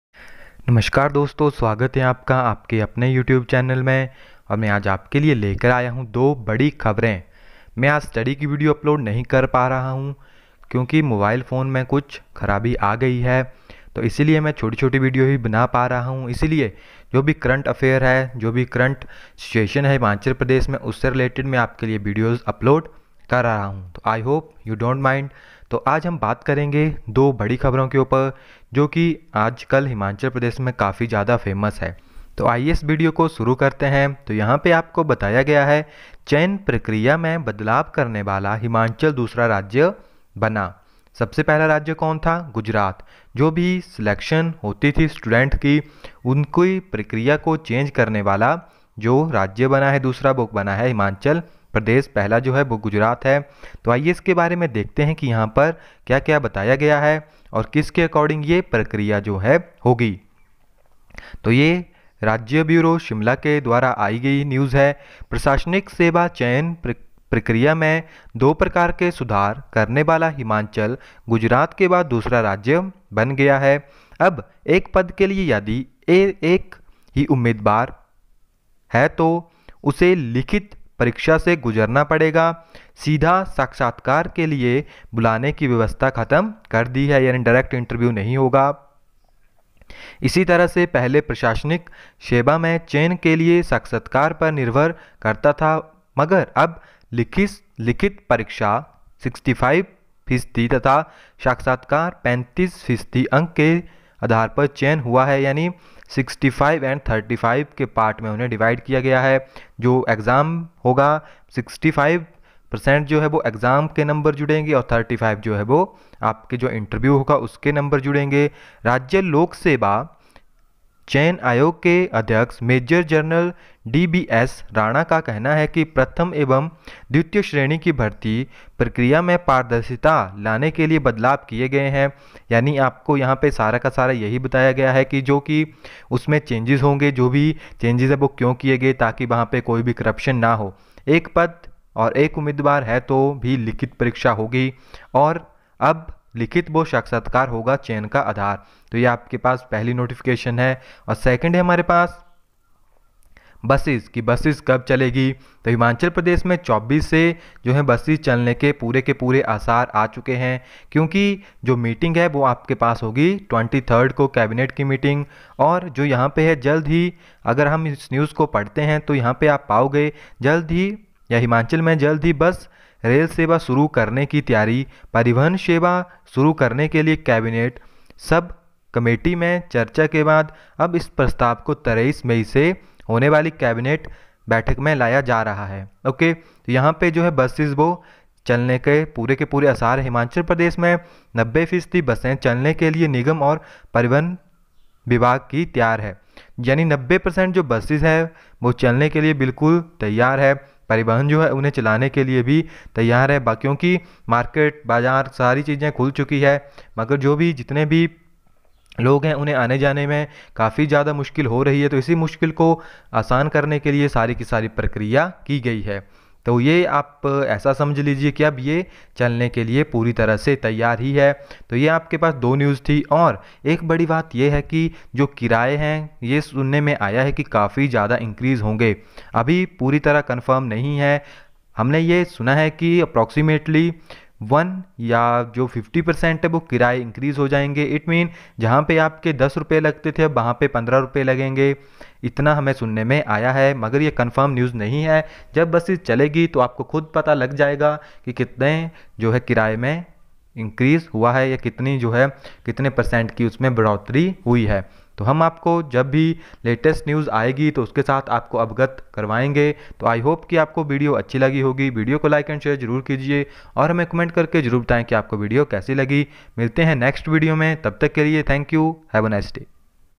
चयन प्रक्रिया में बदलाब//हिमाचल सरकार का बड़ा फैंसला//Latest Himachal pradesh News //current hp news
चयन प्रेक्रिया को लेकर हिमाचल सरकार का बड़ा फैंसला//Latest Himachal pradesh News //current hp news